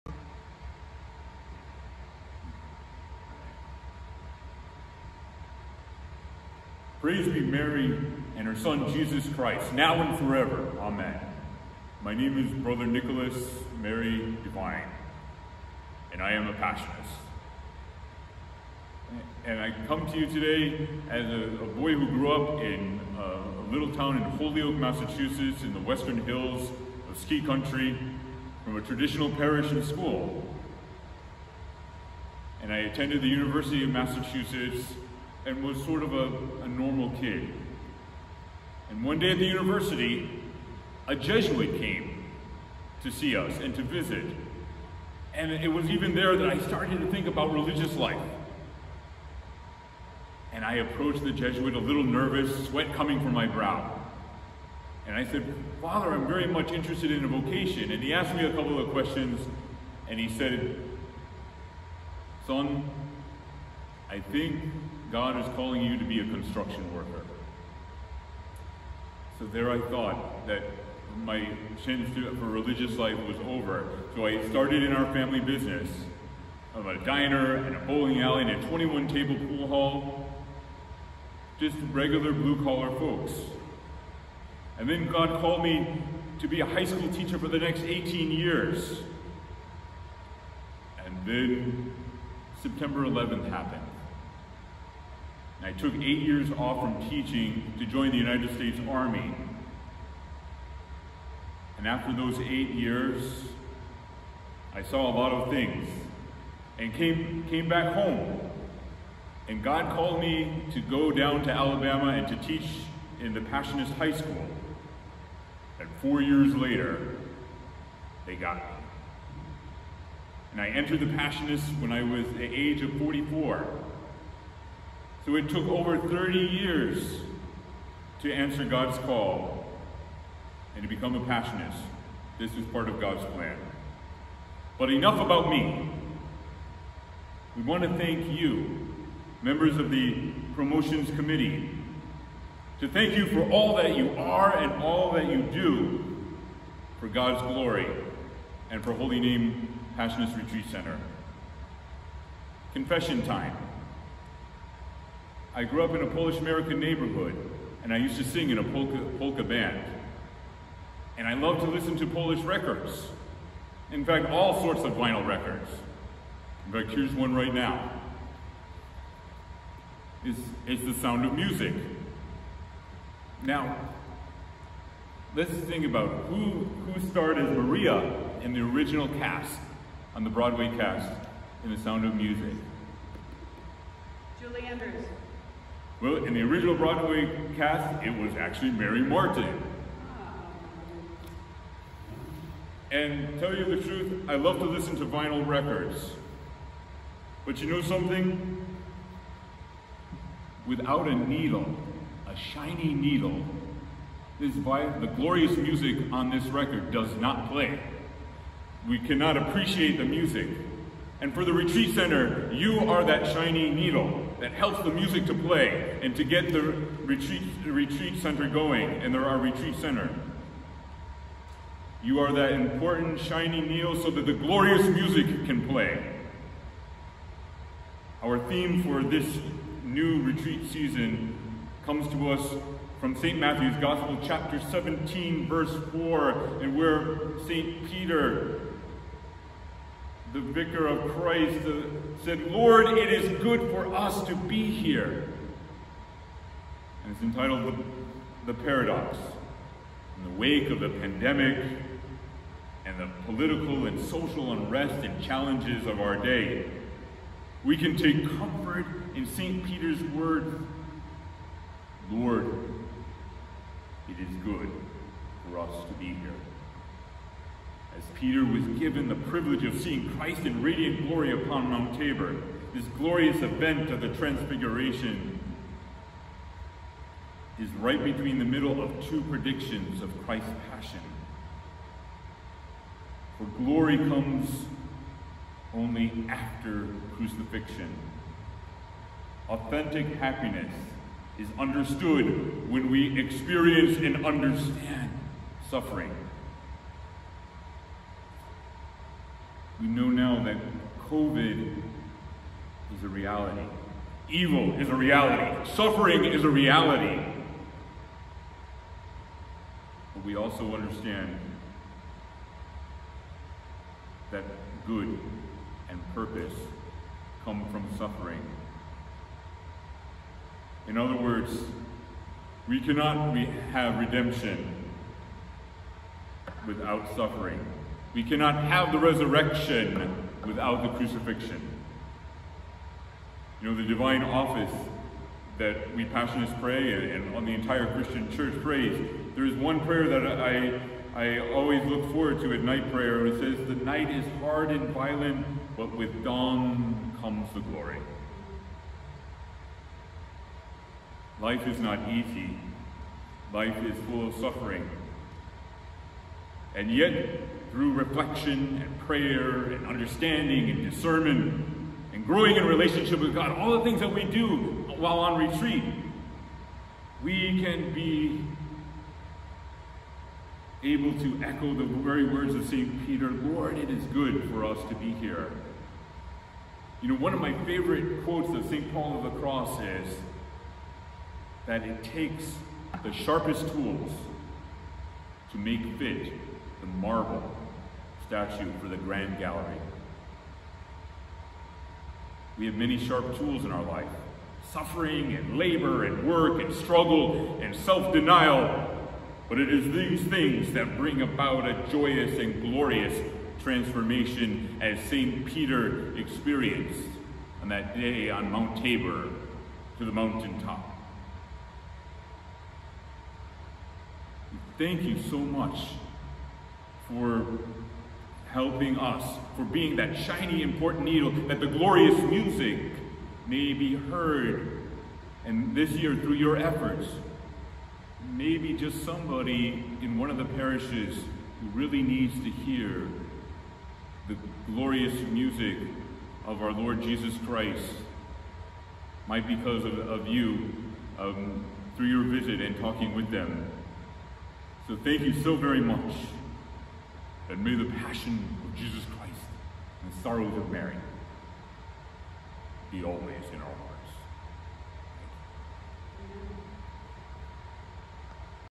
Lord, it is good for us to be here.
Director of Ministry, Nicholas Divine, C.P. introducing Holy Name Passionist Retreat Center's Retreat Theme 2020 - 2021